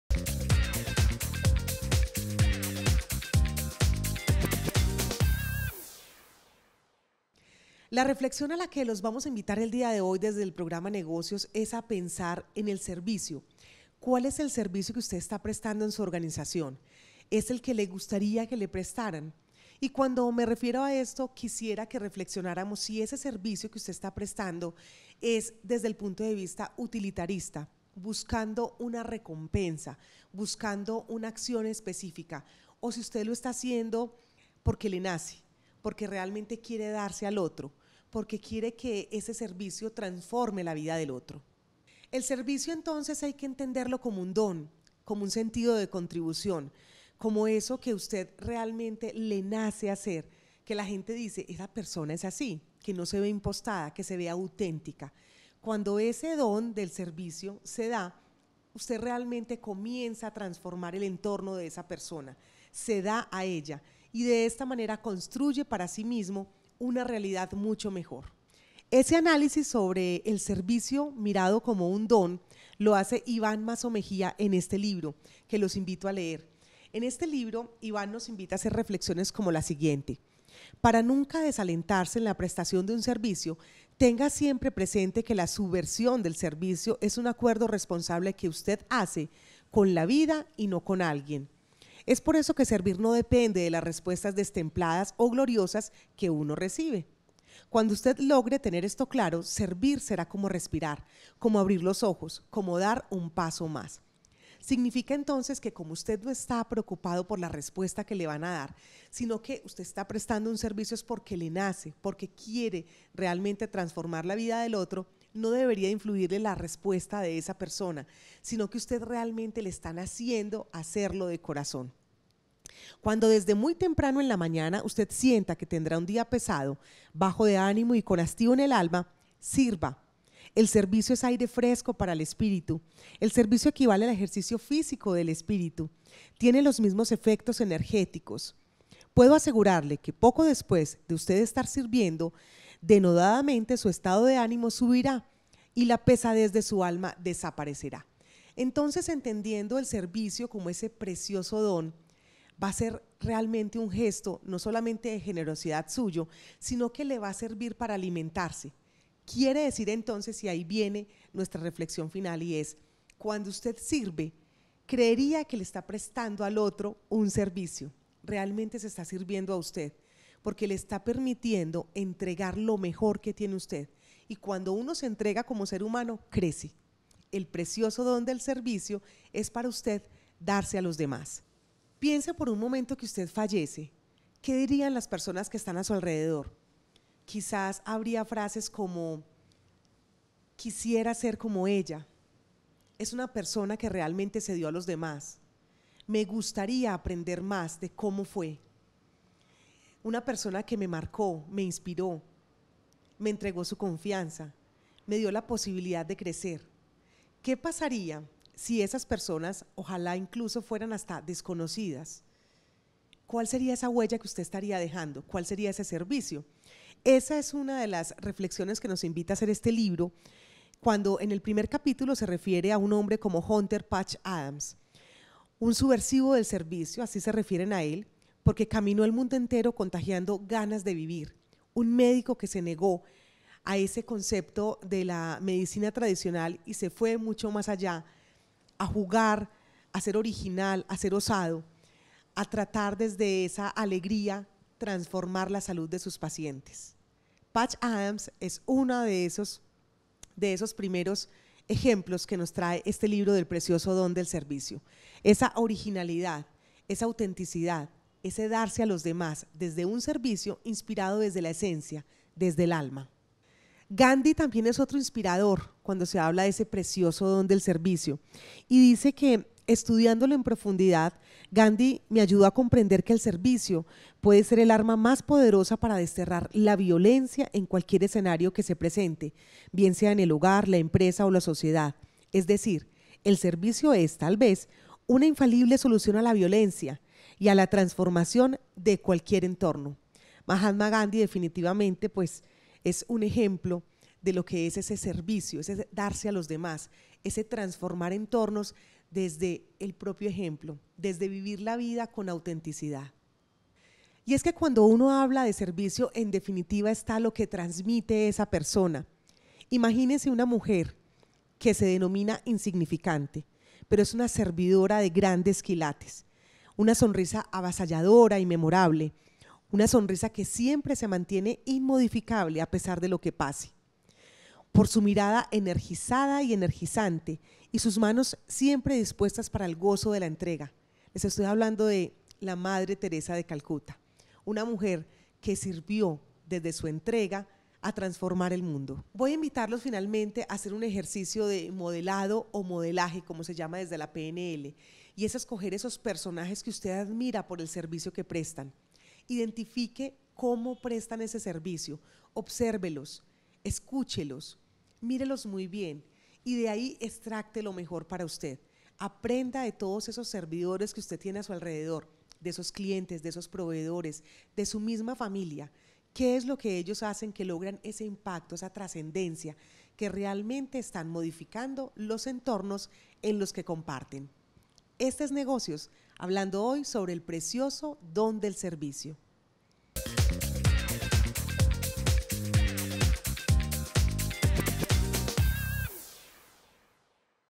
Libro el precioso don del servicio de Ivan Mazo Mejia -[Negocios en Tm] ®
@PaolaRuedaLopez invita a leer el libro El Precioso don del servicio, del conferencista y creador de la Escuela de la Riqueza Iván Mazo Mejía @IvanMazo